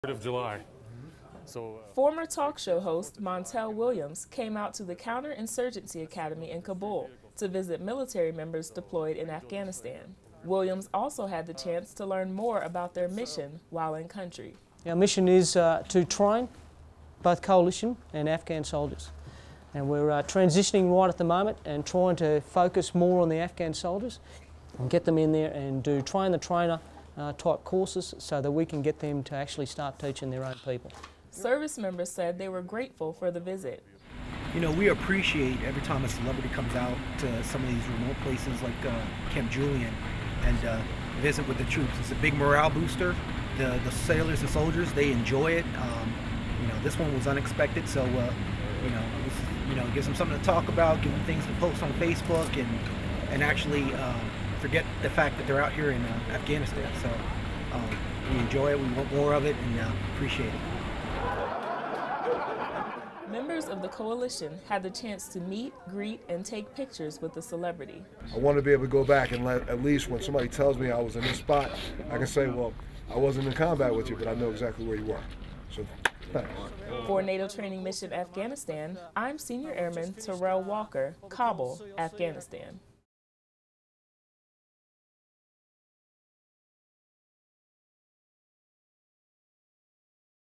Montel Williams Visit to COIN Academy
Former talk show host Montel Williams made a visit to the Counter Insurgency Academy located in Kabul, Afghanistan. He came out to visit military members and to learn more about NATO Training Mission Afghanistan.